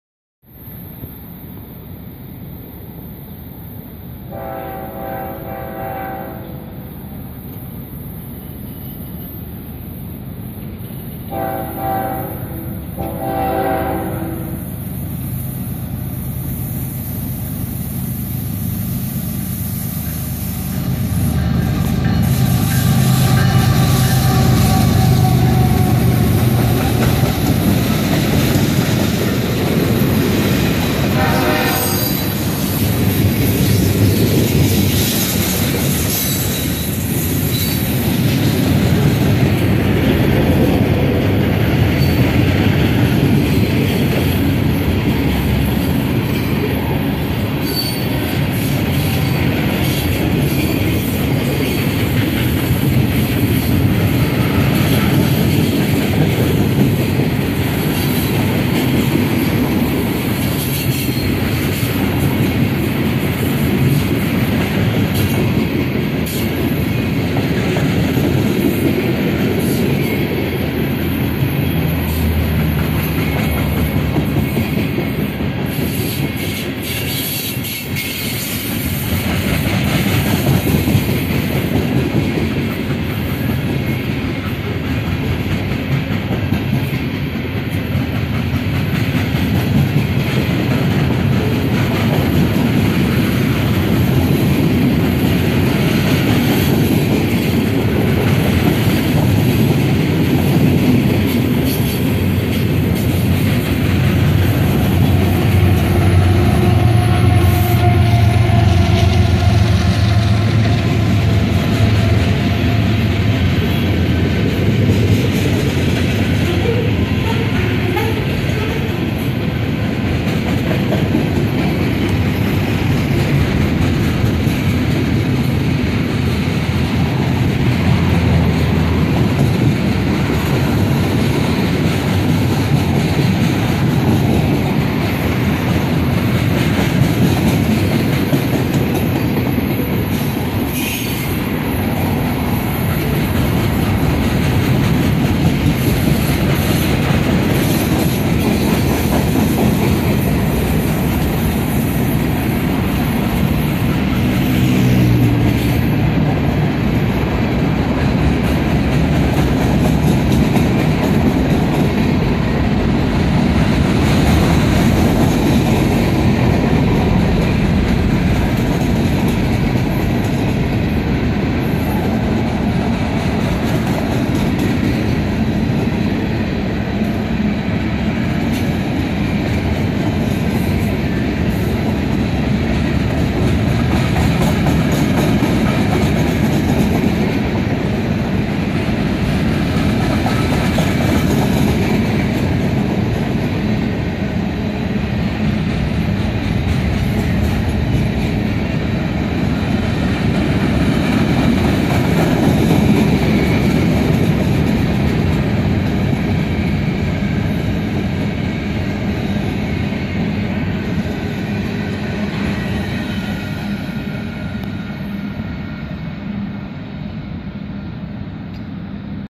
De nuevo la rompe el DROIR con 514 ejes, 121 Carros, 5 locomotoras en formación 3+2 trenesote NOAS_5
Buenas raza aquí les dejo un vídeo de un tren de Ferromex Rumbo al Sur, se trata del DROIR Directo Romo-Irapuato con 121 carros y 5 locomotoras, en esta ocasión la locomotora Líder fue una ES44AC la 4846 FXE, en múltiple traía una AC4400CW la 4522 FXE, Remolcada venía una chulada de máquina de la vieja escuela una Super 7R la 3735 y Remotas dandose beso venian una SD70ACe la 4123 FXE y otra ES44AC la 4697 FXE, este tren al Sur con 514 ejes lo grabe en el kilómetro # 414 de la Línea A en el Distrito de León, en el municipio de León, Guanajuato. México Saludos Gente